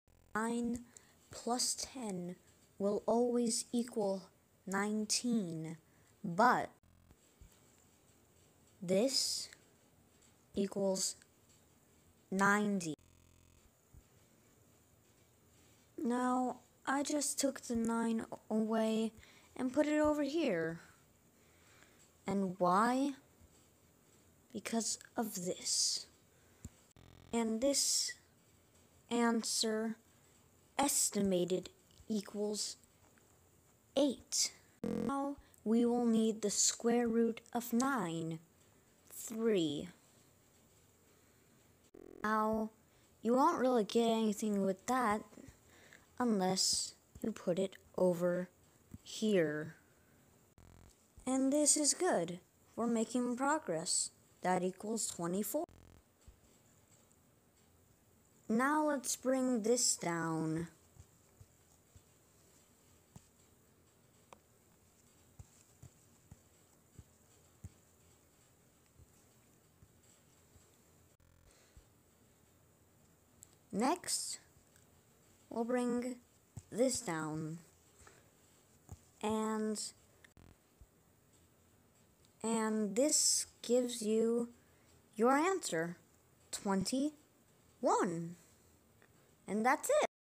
9+10=___
9 plus ten equals nineteen but nine times ten equals ninety and the equal sign turned on its side is an eleven divide the eleven into ninety u get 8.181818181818 and so on... But estimated u get 8. Now we will need the square root of nine 3x3 take the ×3 and put it next to the estimated 8. Its 8×3 ok thats 24. Now take the 1 in 10 to it and turn it side ways u get 8×3=24--. Now take the other 3 from"the square root of nine 3x3" and put it next to it. 8×3=24--3= and now what do u know its 23